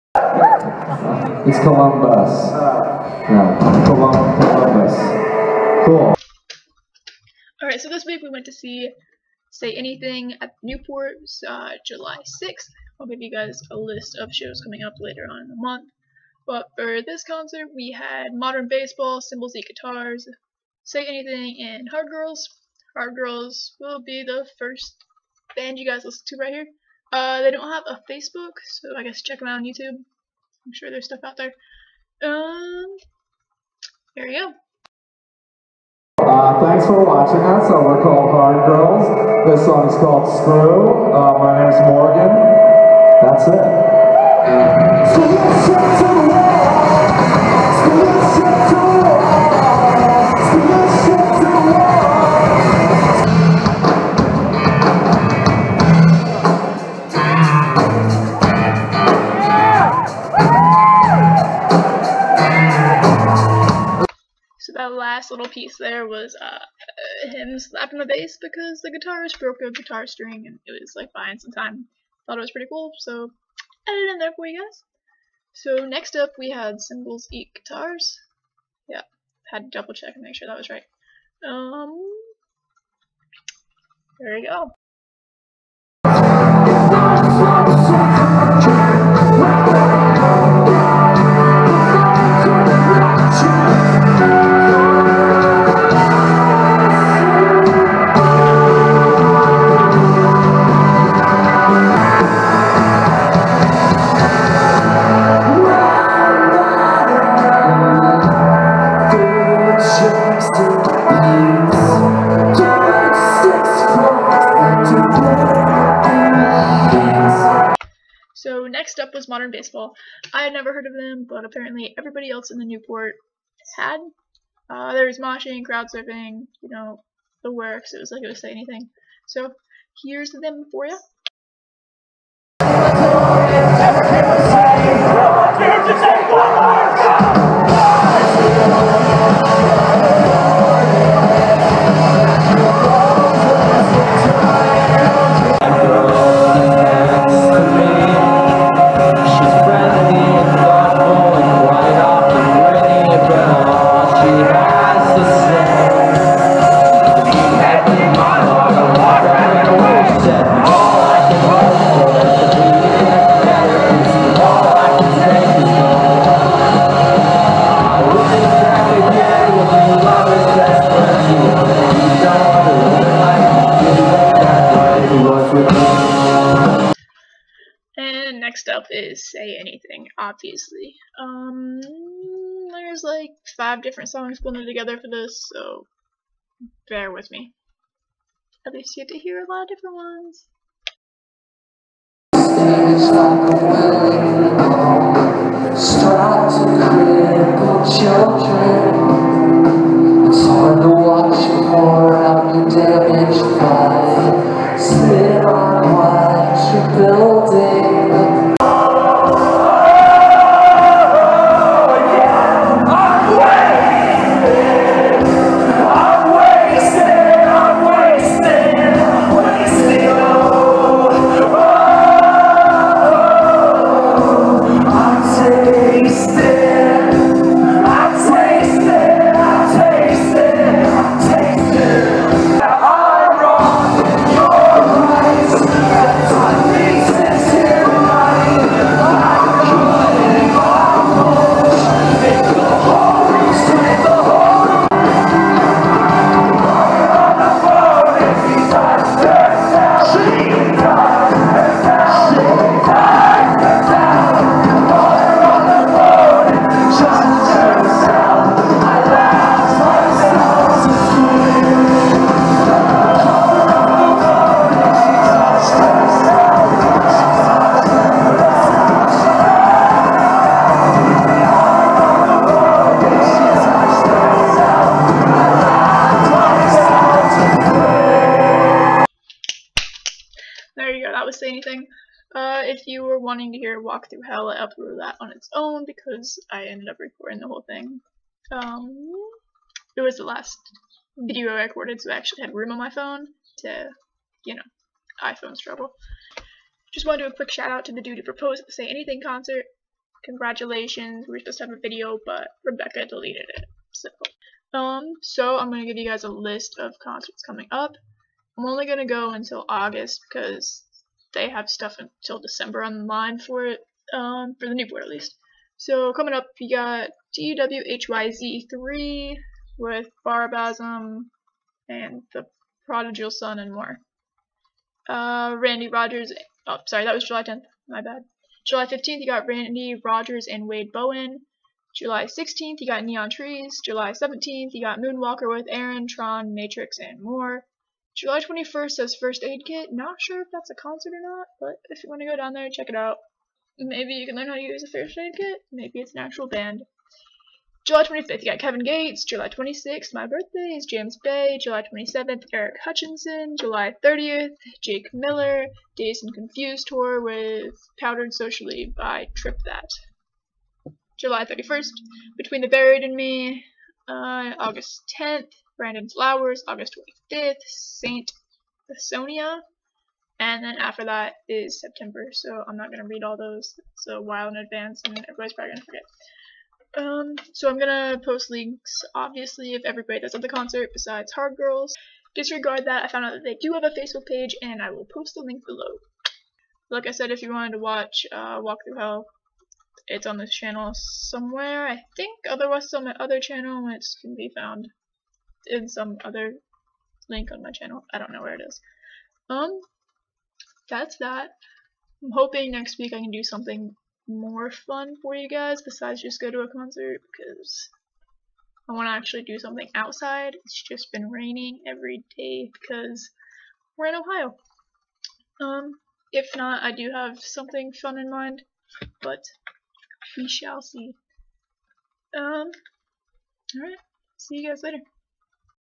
Say Anything Concert
July 6th at Newport Music Hall was Say Anything, Modern Baseball, Cymbals Eat Guitars, and Hard Girls. Below are links to their facebook pages as well as a link to Newports page so you can take a look at upcoming concerts.